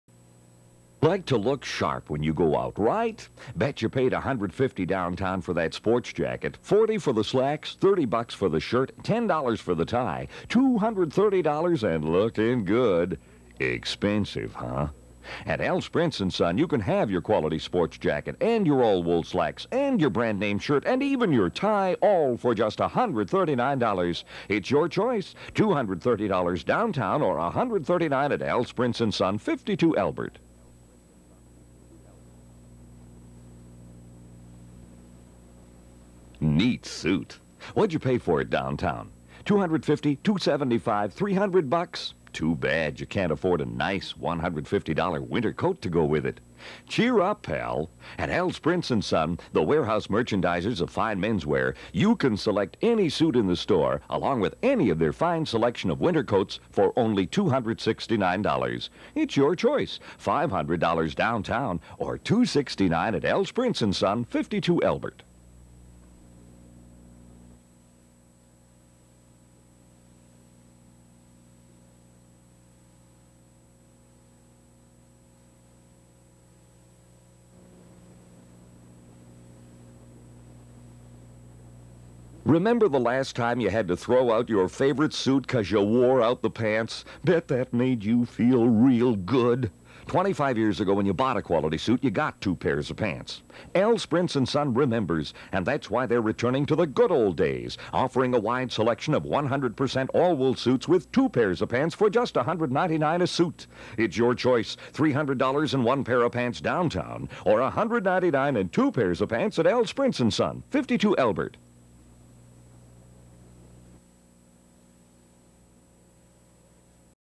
Al Sprintz TV spot
Funny tv commercial In memory of my good friend Marvin Meltzer who produced these spots.